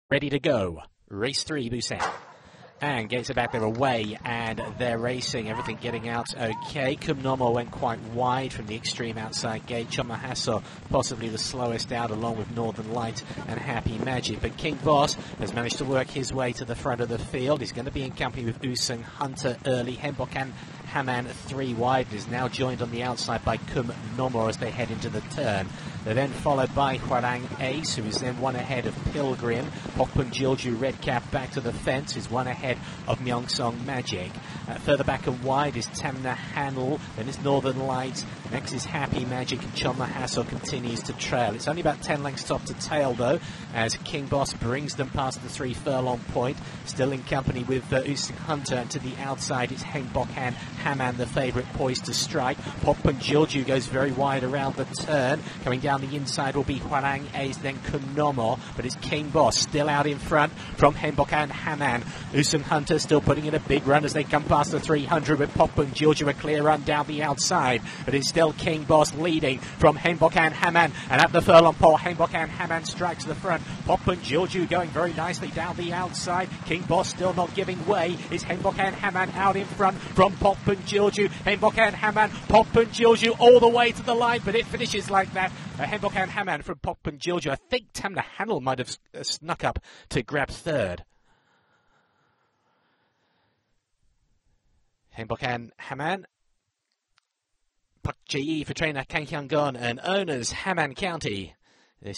(Busan) 2022.06.19 R3 Tongue-twisting finish as Haengbokan Haman denies Pokpung Jilju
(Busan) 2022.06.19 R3

W: Haengbokan Haman
J: Park Jae-i
T: Kang Hyoung-gon
O: Ham An County